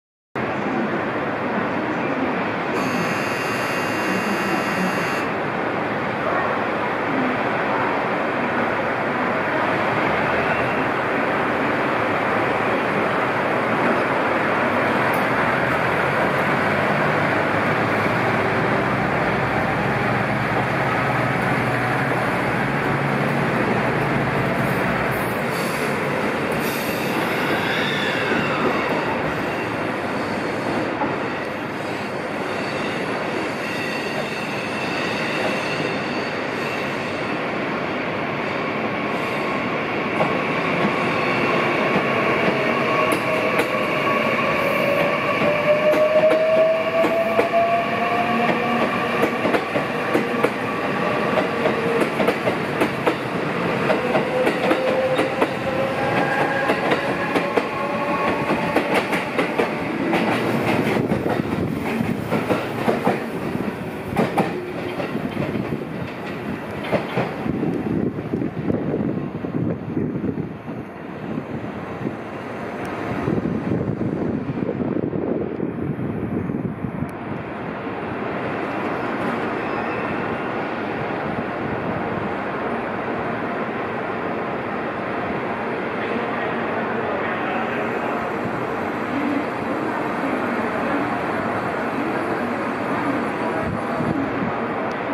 JR東海の新型車両！315系C4編成 快速 中津川行き 中央本線 名古屋駅
場所：中央本線 名古屋駅
編成：315系C4編成
種別：快速 中津川行き
列番：2735M
撮影：2022年4月10日